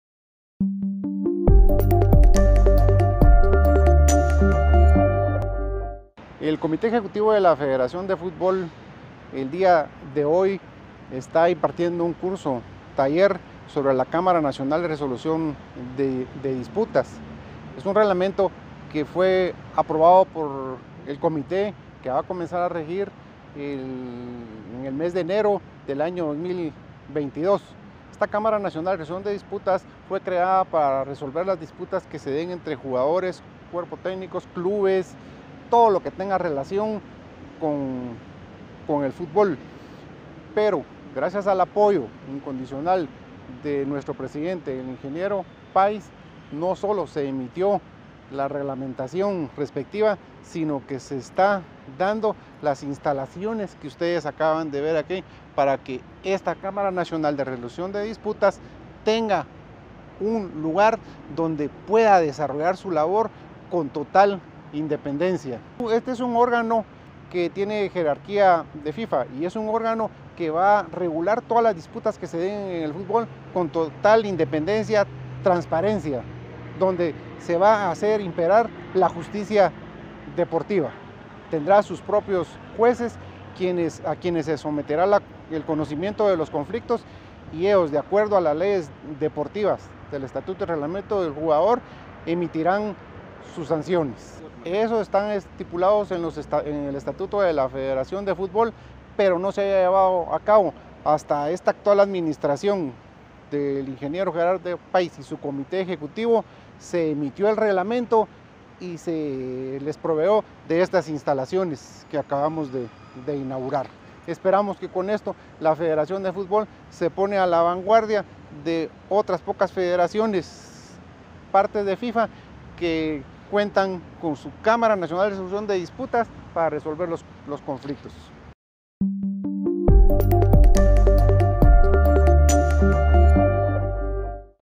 FEDEFUT INAUGURA CÁMARA NACIONAL DE RESOLUCIÓN DE DISPUTAS
Se inauguró la oficina de la CÁMARA NACIONAL DE RESOLUCIÓN DE DISPUTAS que será implementada en nuestro país el próximo mes de enero 2022.